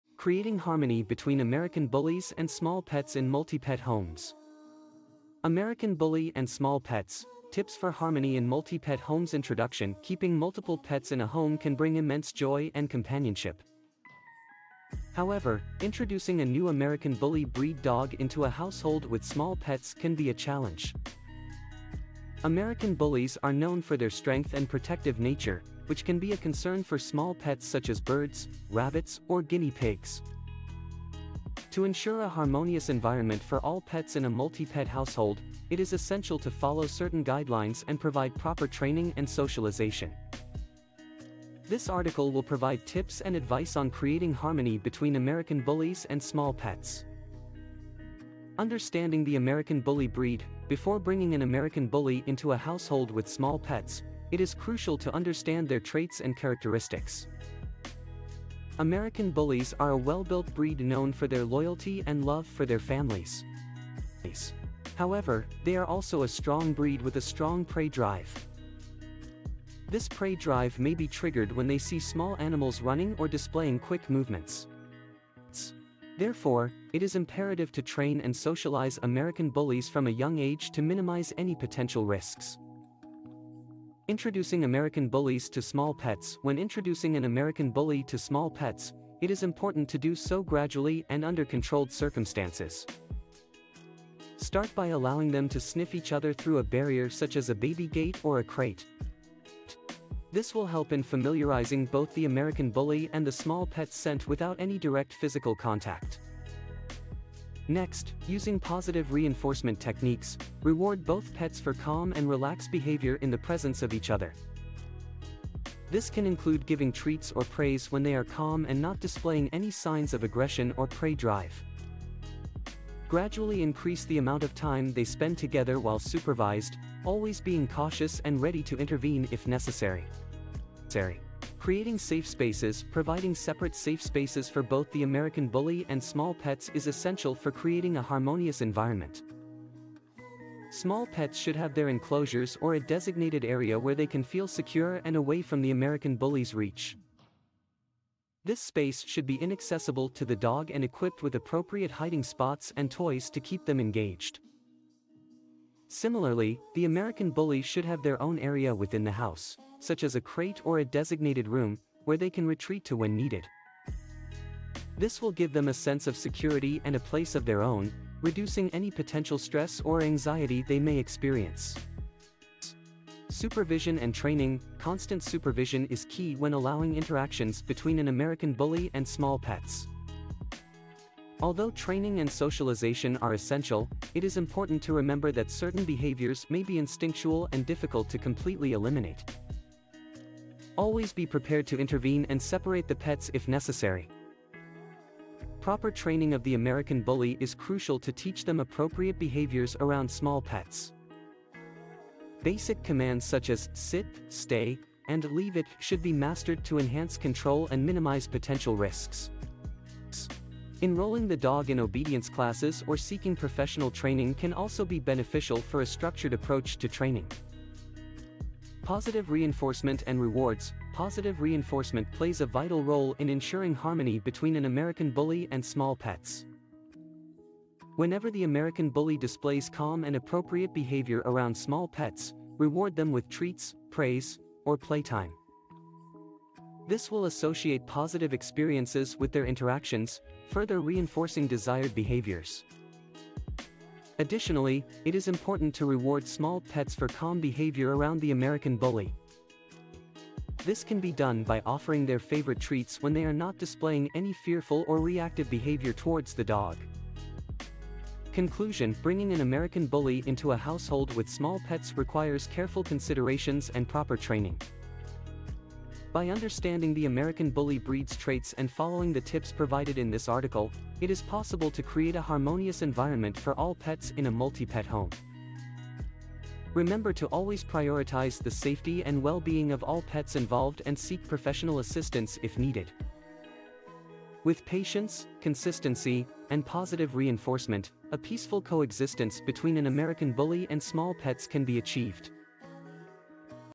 Creating Harmony Between American Bullies and Small Pets in Multi Pet Homes
American Bully and Small Pets: Tips for Harmony in Multi-Pet Homes

Introduction:

Keeping multiple pets in a home can bring immense joy and companionship  However, introducing a new American Bully breed dog into a household with small pets can be a challenge  American Bullies are known for their strength and protective nature, which can be a concern for small pets such as birds, rabbits, or guinea pigs  To ensure a harmonious environment for all pets in a multi-pet household, it is essential to follow certain guidelines and provide proper training and socialization  This article will provide tips and advice on creating harmony between American Bullies and small pets
00:00 Creating Harmony Between American Bullies and Small Pets in Multi-Pet Homes